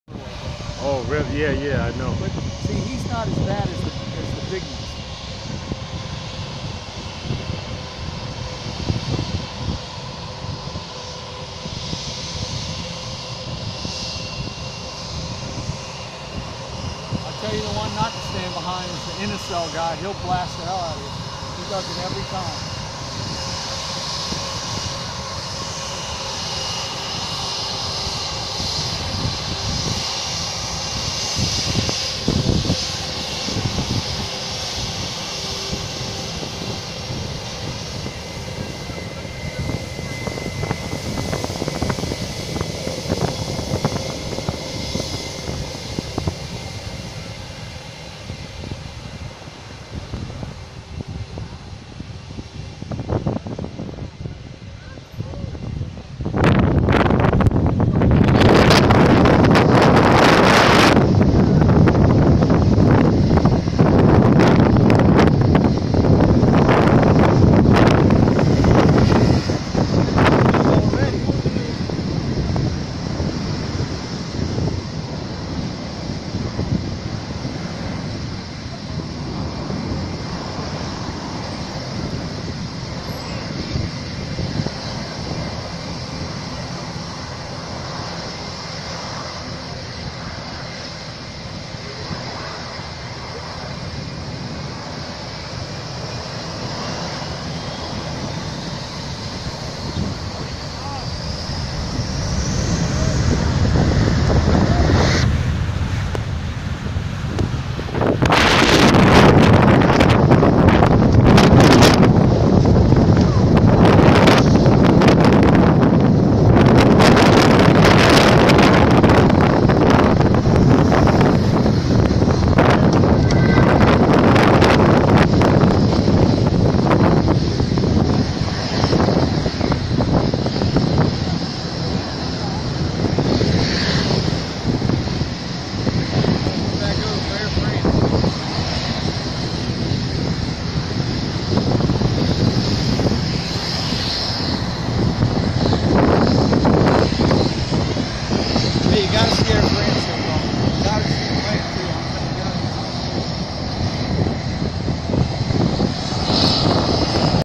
St Maarten Smx Jet Blue A321 Take off Jet Blast Nov 5th 2015
Smx runway . This particular Jet runs back and forth on this route to JFK International in New York everyday  .